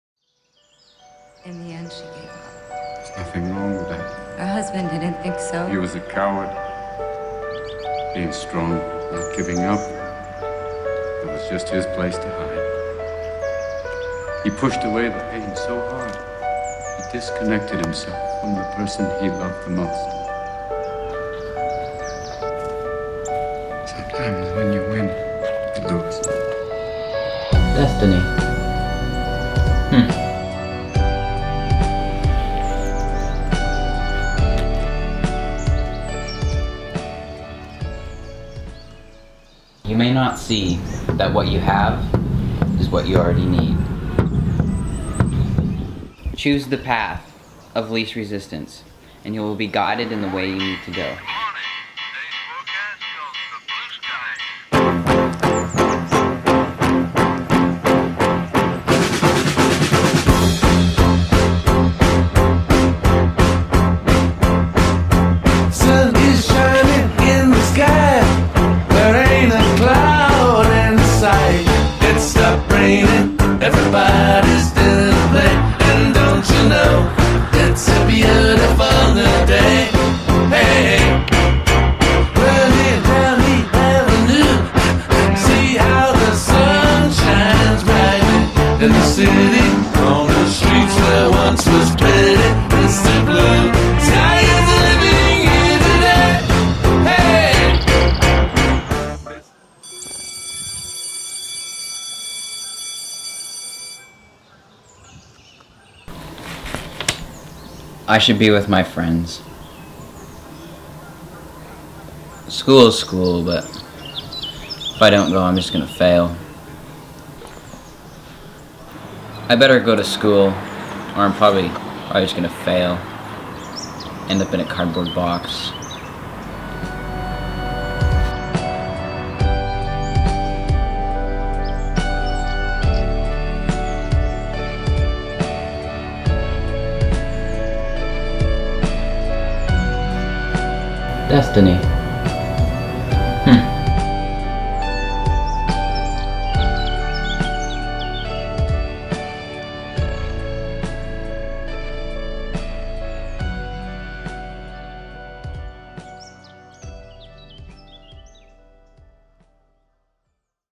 Ethan Ouellette Skills Canada Regionals Entry
First place Skills Canada Regional Competition Video by Timberline Secondary students.